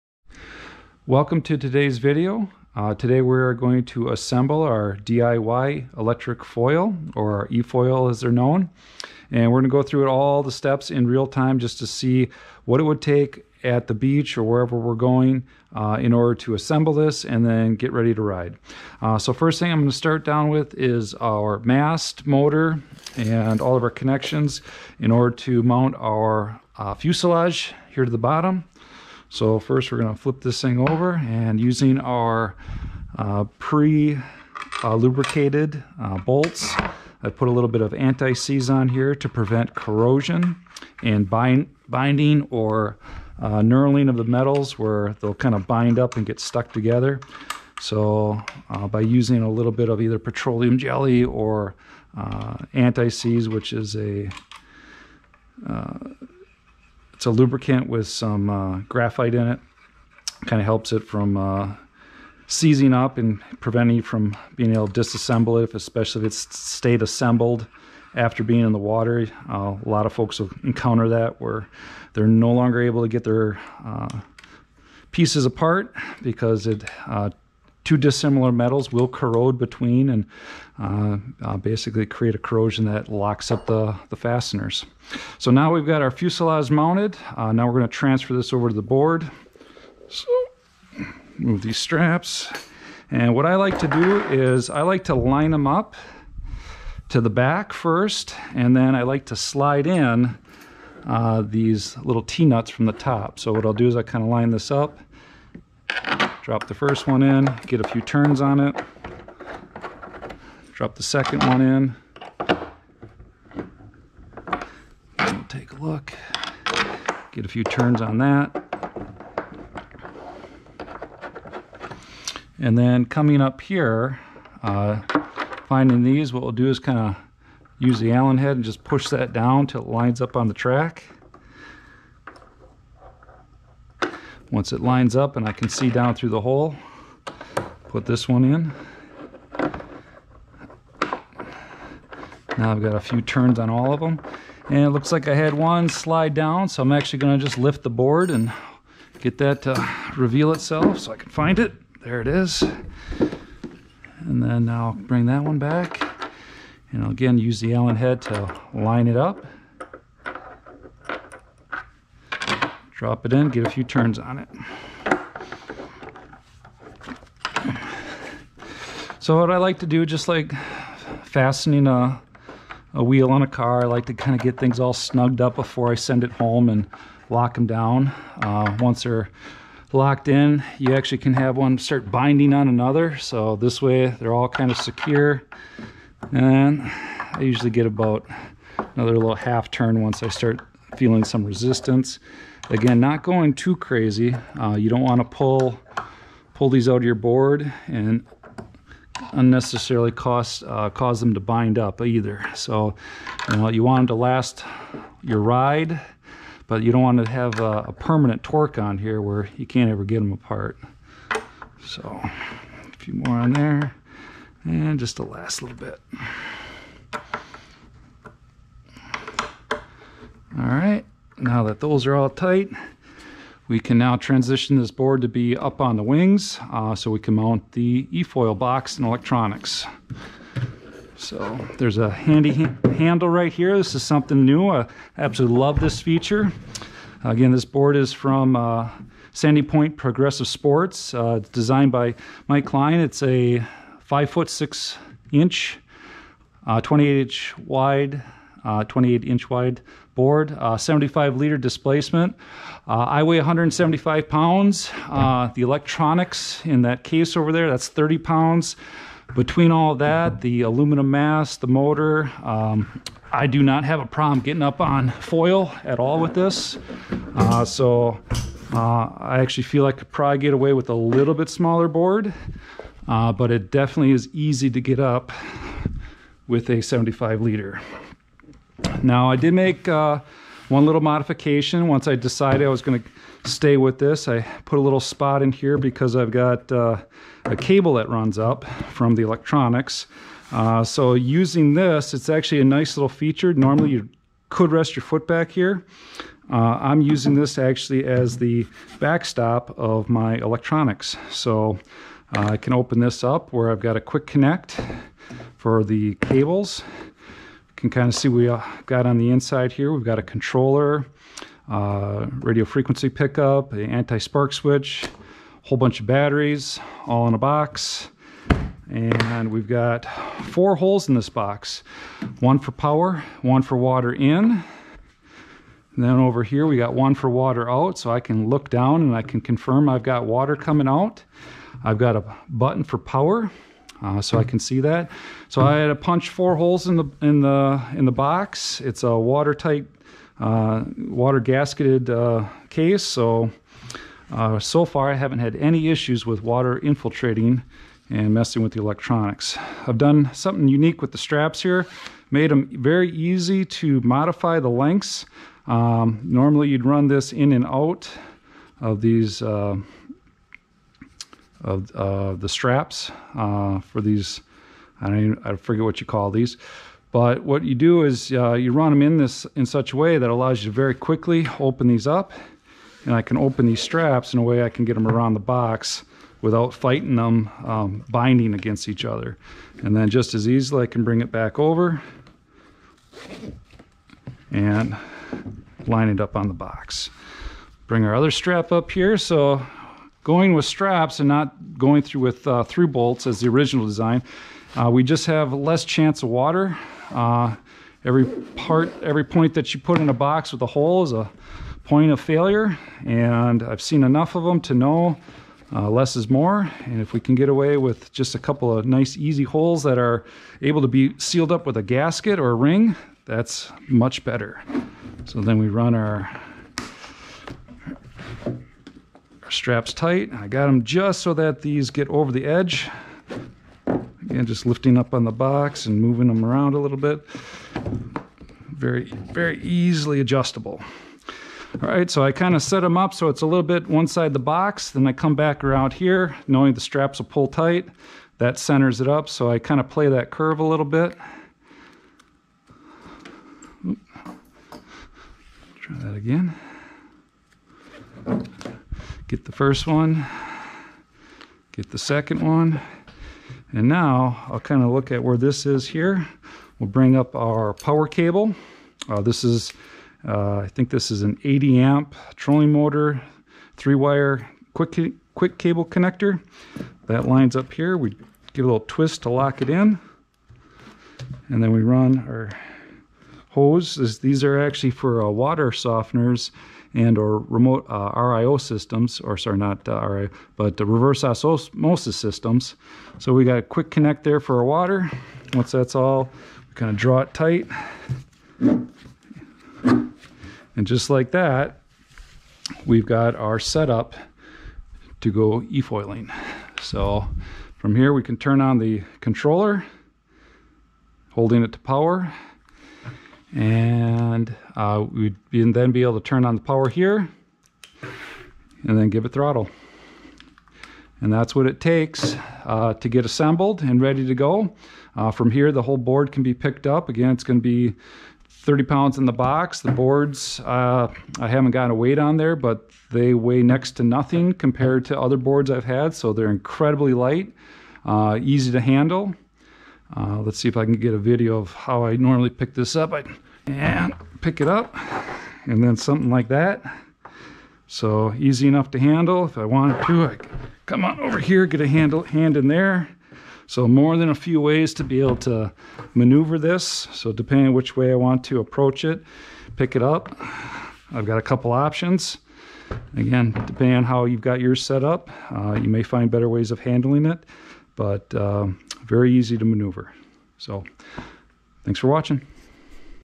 DIY Efoil Assembly
This video shows how to assemble a DIY Efoil made from Flipsky electronics, Slingshot hydrofoil, and Sandy Point Progressive Sports Foil Board.